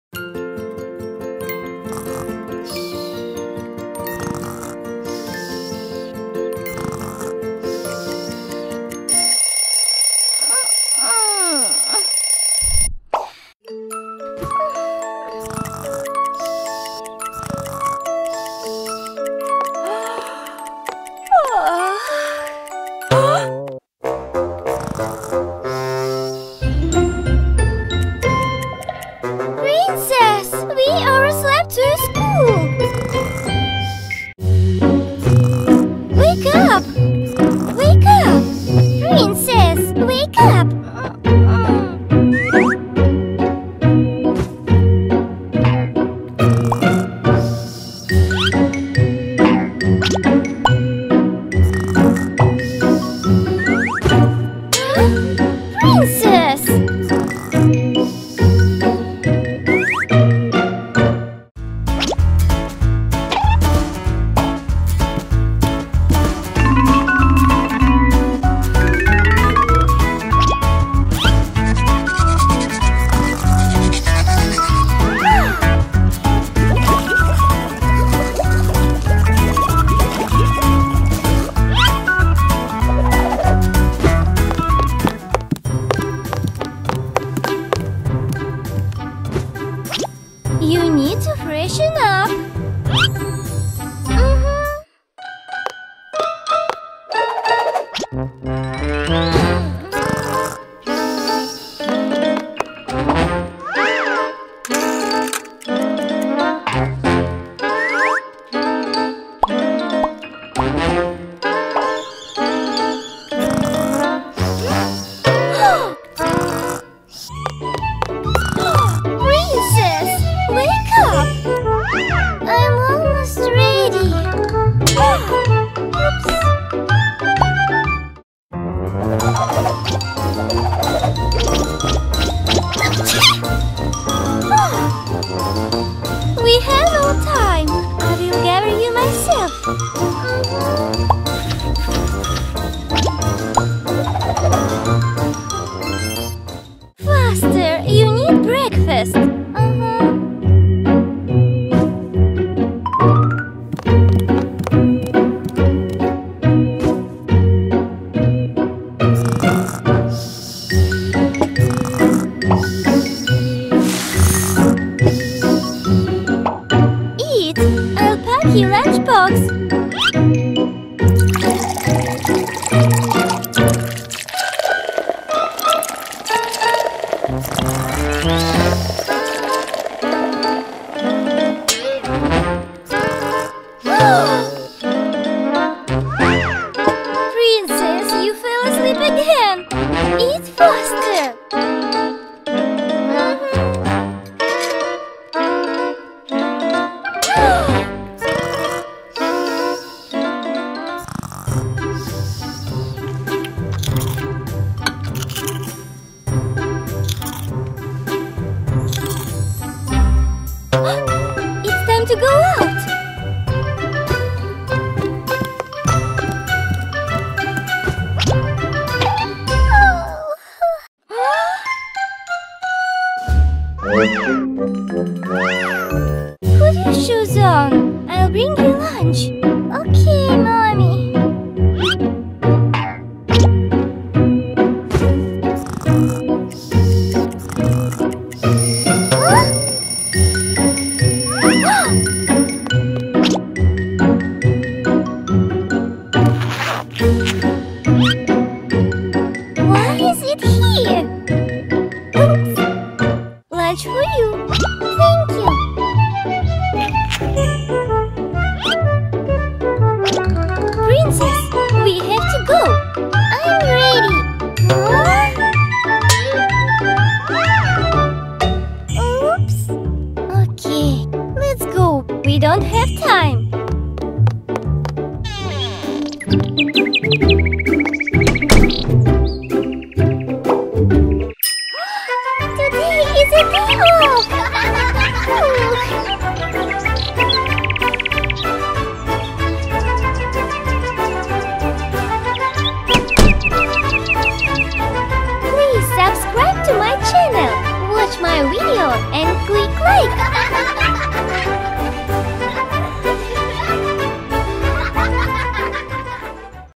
Alice goes to preschool puts on shoes and brushes her teeth
Alice as a kids hurry up to school puts on her shoes, has breakfast and brushes her teeth. Alice Smile and Mommy Pretend Play that they Late for School Morning Routine and have to do their morning routine for kids.
Watch our funny video for kids !